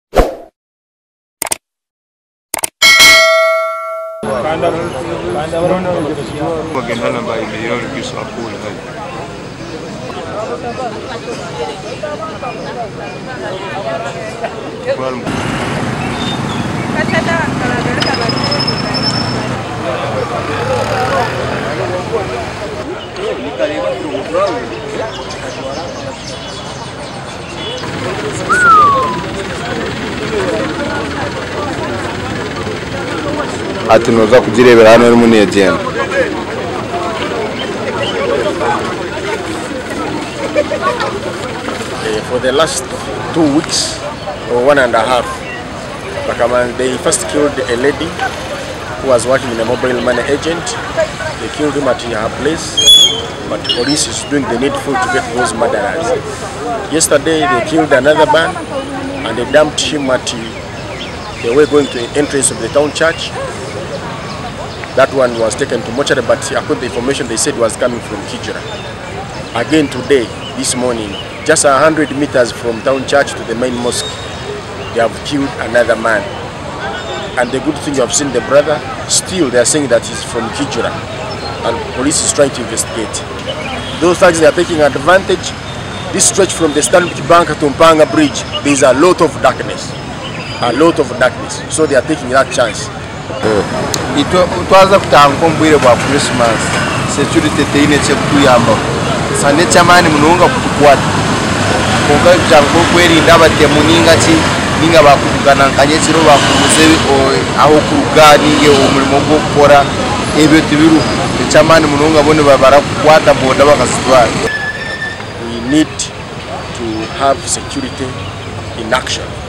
TWO PEOPLE KILLED FROM THE SAME SPOT IN JUST TWO DAYS IN FORT PORTAL
Another man has been found dead almost on the same spot where another unidentified man was killed yesterday in Fort Portal.
The body of an unidentified man was discovered today morning by passers-by near Fort Portal main mosque just a few meters from where another man unidentified as of today by police was found near St Charles Lwanga catholic church.
Today’s suspected murder case makes the number to three people killed in a space of one week with two people killed in just two days in Fort Portal.
This has left leaders and security authorities pondering for answers on what could be catalyzing the murderers in the newly elevated tourism city.
Others are also attributing the murders to the lack of cameras and security lights on the spot where the bodies are being found saying cameras would have helped in getting the murderers.
Muhumuza however challenges the courts of law to stop releasing people that are known to be hardcore criminals as one way of stopping incidents like these.
Police had not yet released a statement to the same.